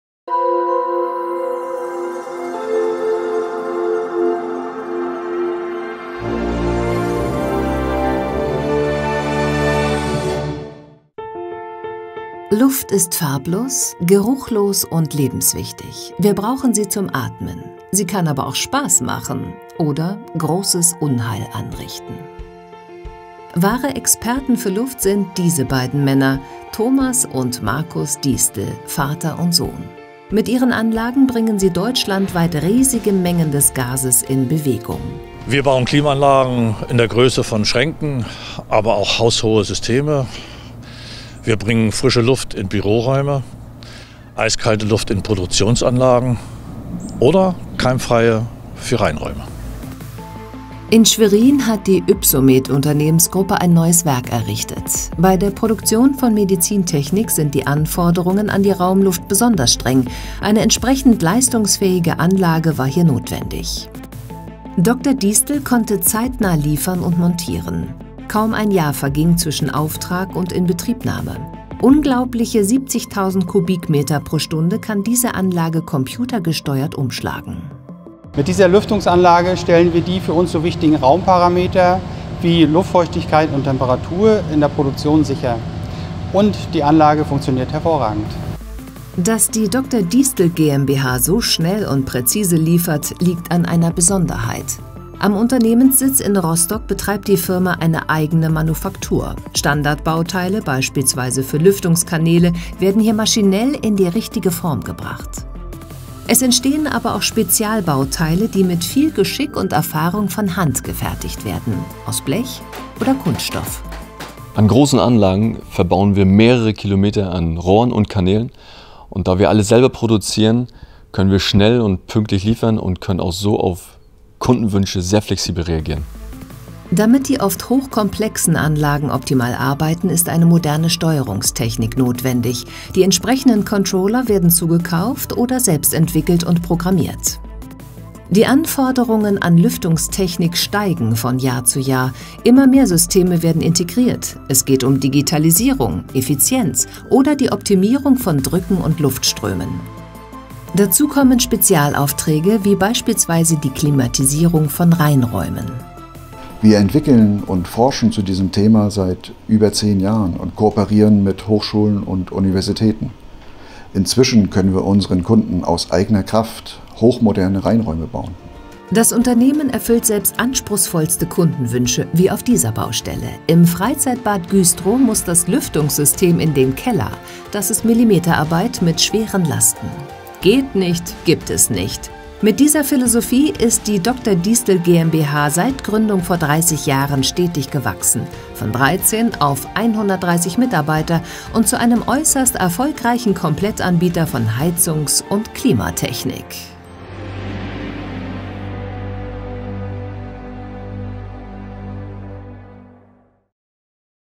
Unternehmen des Jahres Mecklenburg-Vorpommern: Dr. Diestel GmbH
Seit Corona ist ein gutes Klima wichtiger denn je. Das Unternehmen des Jahres in Mecklenburg-Vorpommern, die Dr. Diestel GmbH aus der Hansestadt Rostock, setzt diese Erkenntnis schon seit bald 30 Jahren in gute Produkte um. Die 130 Mitarbeiter sind gefragt, wenn es um Lüftungs-, Klima-, Kälte-, Reinraum-, Regelungs- oder Elektrotechnik geht. Meistens betreffen die Aufträge gleich mehrere dieser Tätigkeitsfelder. Genau durch diese Systemverantwortlichkeit zeichnet sich das Unternehmen aus. „Alles aus einer Hand“ ist das Motto. Der Umsatz lag im vergangenen Jahr bei
18,2 Millionen Euro.